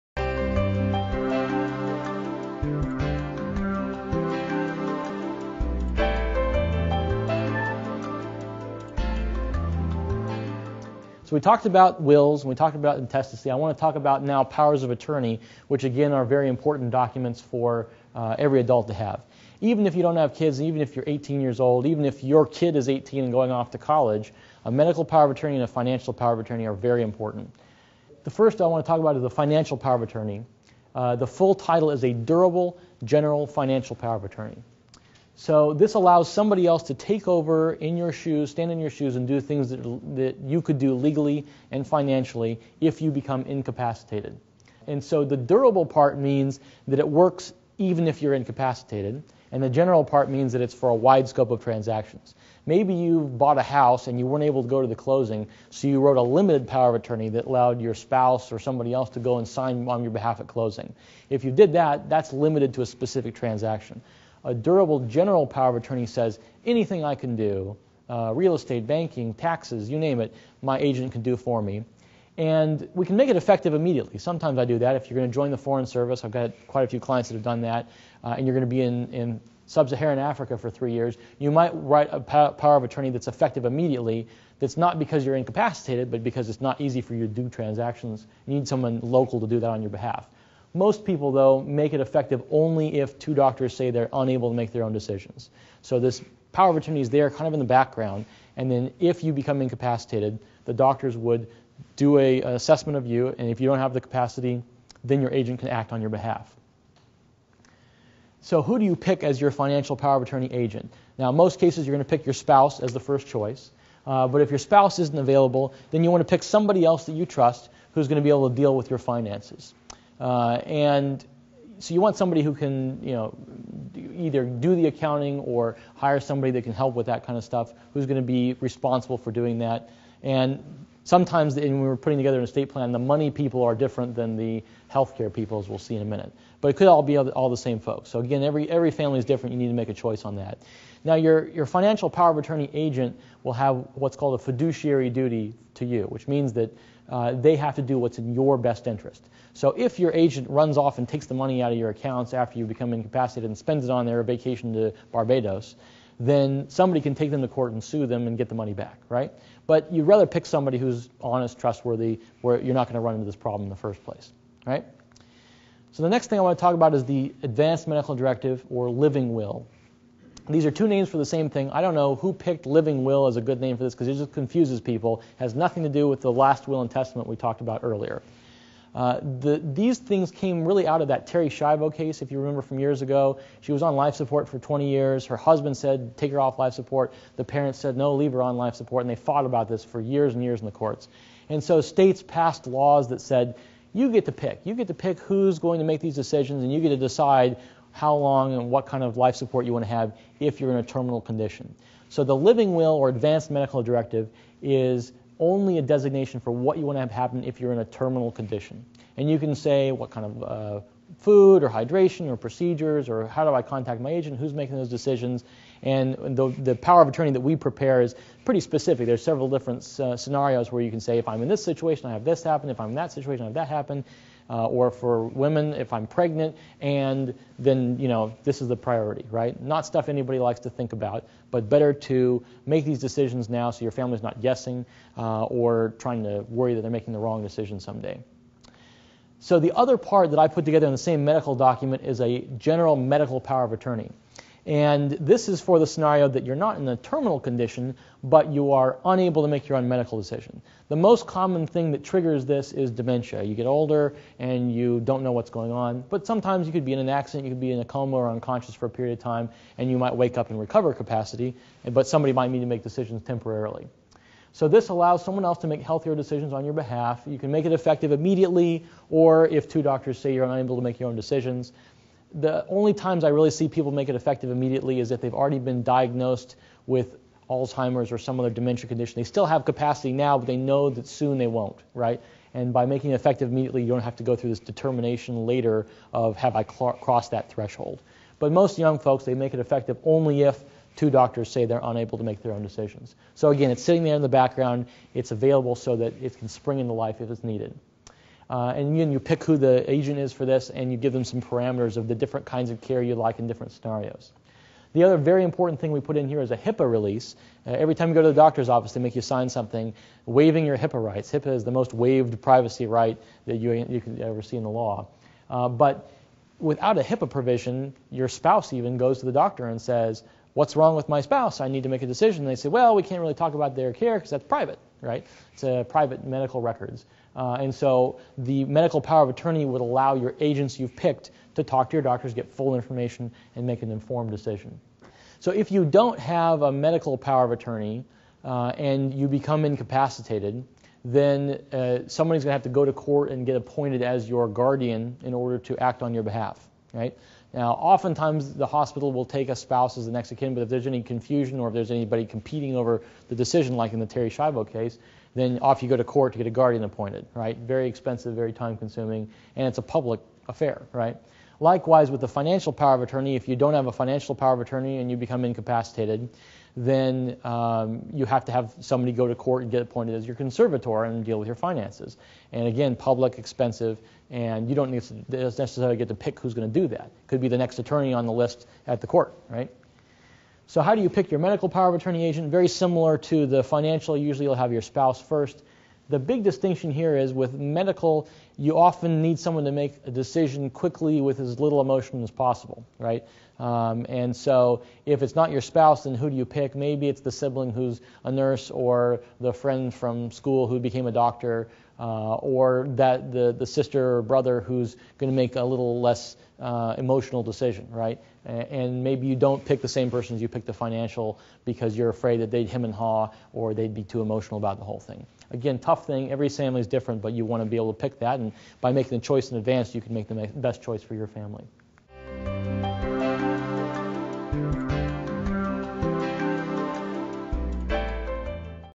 Medical and Financial Powers of Attorney
Learn about Medical Powers of Attorney, Advance Medical Directives, and Durable General Financial Powers of Attorney for incapacity planning.